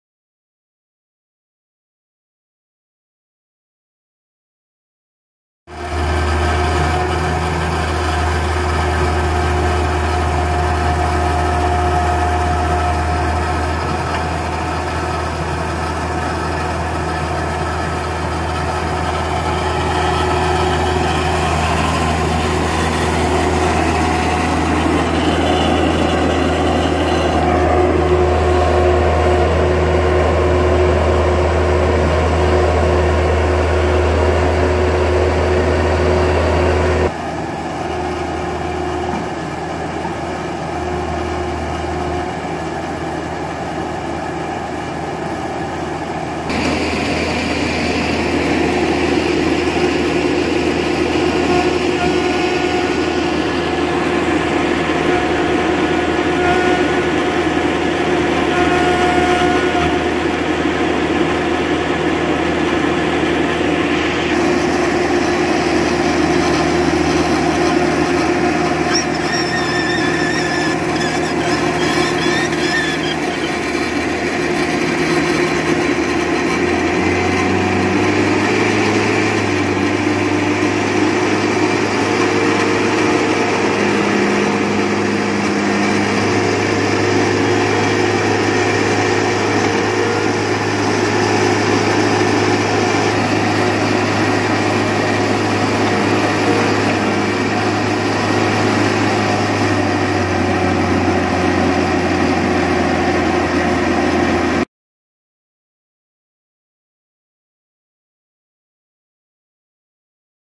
2006 Vermeer D7x11 Series II - Equipment Demonstration
Reference #24638 – 2006 Vermeer D7x11 Series II Directional Drill:

-1,757 hours

Pipe:
(30) Drill Rods.

Accessories:
Bore Head.

Additional Information:
NOT INCLUDED - Trailer with Tank available at additional cost.

The inspection report for this listing has been flagged with a disclaimer informing potential buyers of the fact that the seller supplied the inspection report. HDD Broker highly recommends that an inspection be carried out by a qualified mechanic from an independent third party company.